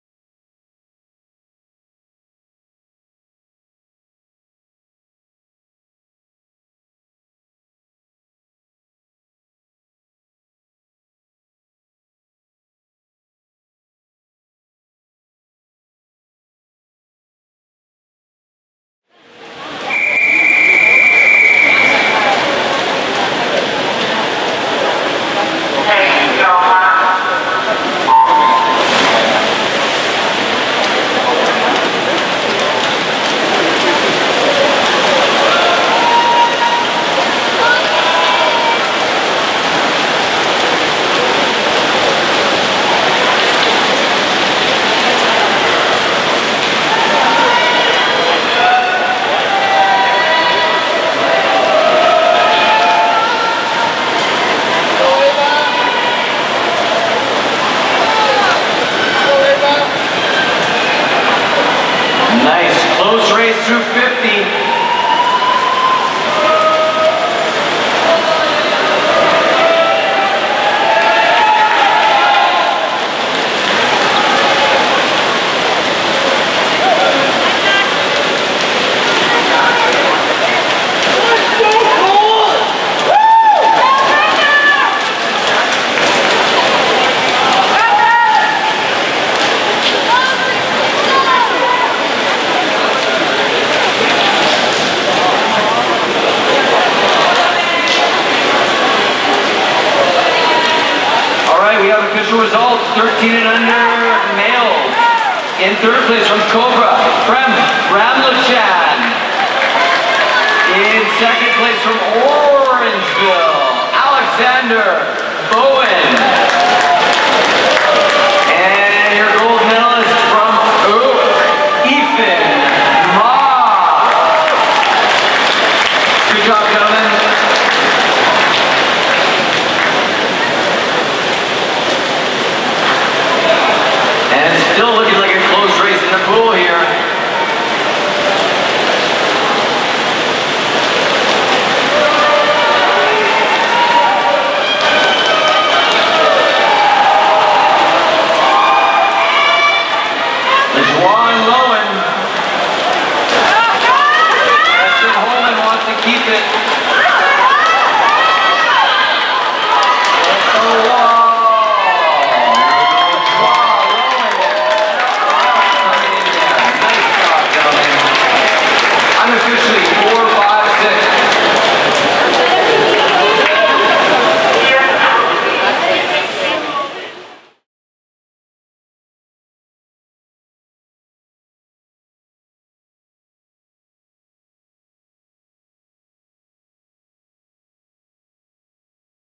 Griffin Lajoie TSC 200 Back Finals
2018 Ontario Summer Provincials
Etobicoke Olympium